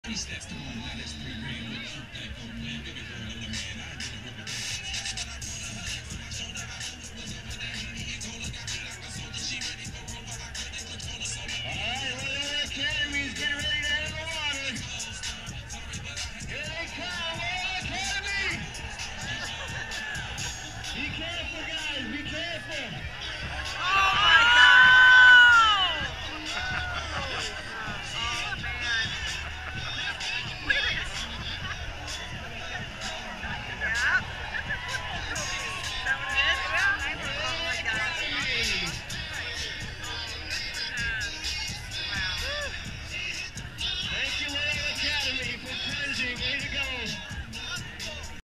Loyola Polar Plunge 2019
Wilmette Loyola Academy takes The Polar Plunge.
Loyola Academy Football State Champions take the Polar Plunge